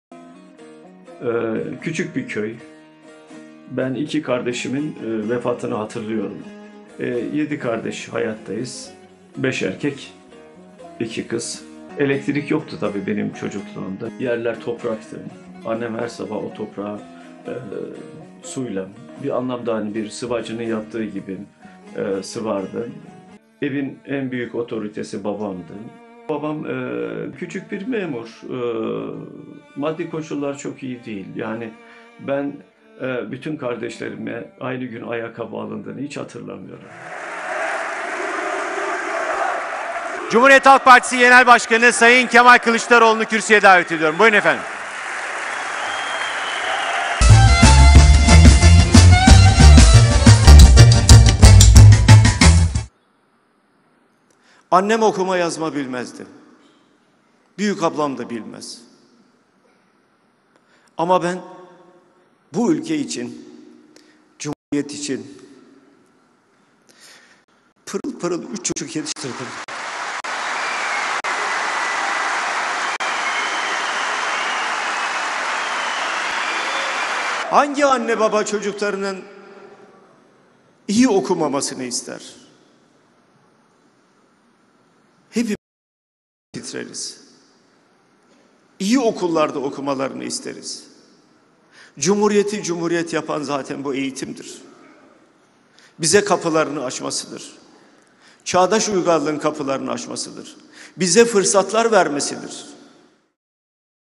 Kılıçdaroğlu'nun duygusal anları
CHP Genel Başkanı Kemal Kılıçdaroğlu, İstanbul Kongre Merkezi'ndeki "Anadolu'nun Kemal'i, Türkiye'nin Başbakanı" belgeselinin gösterimine katıldı. Belgesel gösteriminin ardından konuşan Kılıçdaroğlu, hayatından kesitler anlatırken duygusal anlar yaşadı.